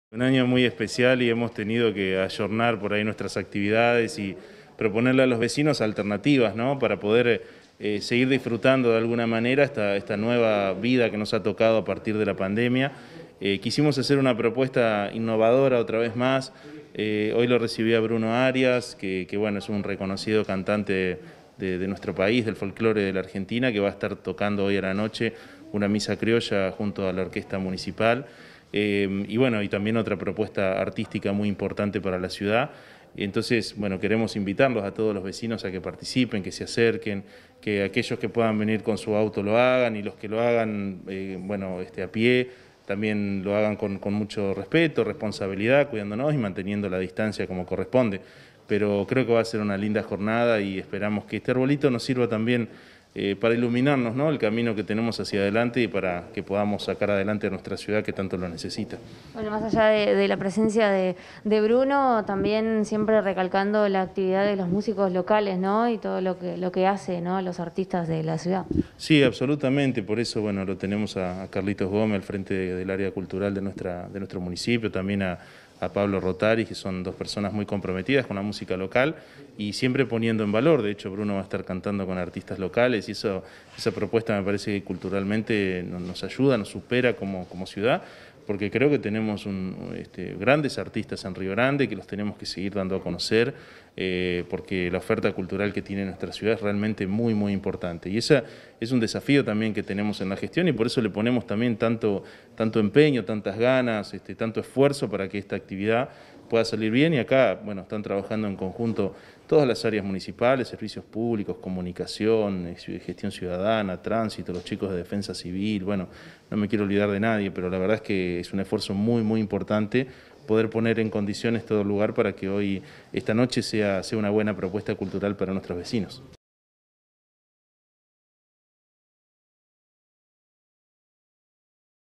AUTOENCENDIDO DEL ARBOLITO DE NAVIDAD EN RÍO GRANDE
Antes de su presentación en el autoencendido, el intendente Martín Pérez recibió a los artistas Bruno Airas y Nicolás Faez Micheloud, quienes se presentaron junto a la orquesta municipal "Kayen".